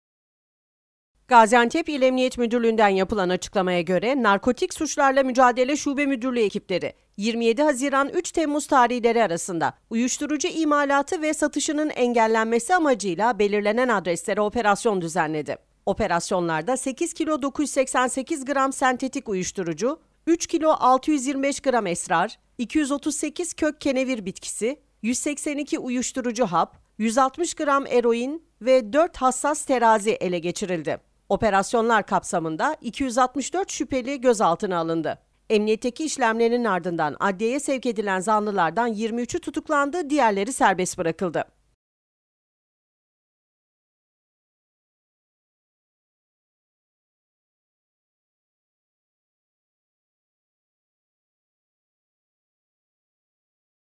Gaziantep Polisinden Dev Operasyon
Gaziantep İl Emniyet Müdürlüğünden yapılan açıklamaya göre, Narkotik Suçlarla Mücadele Şube Müdürlüğü ekipleri, 27 Haziran-3 Temmuz tarihleri arasında uyuşturucu imalatı ve satışının engellenmesi amacıyla belirlenen adreslere operasyon düzenledi.

Operasyonlarda 8 kilo 988 gram sentetik uyuşturucu, 3 kilo 625 gram esrar, 238 kök kenevir bitkisi, 182 uyuşturucu hap, 160 gram eroin ve 4 hassas terazi ele geçirildi.

Operasyonlar kapsamında 264 şüpheli gözaltına alındı.

Emniyetteki işlemlerinin ardından adliyeye sevk edilen zanlılardan 23'ü tutuklandı, diğerleri serbest bırakıldı.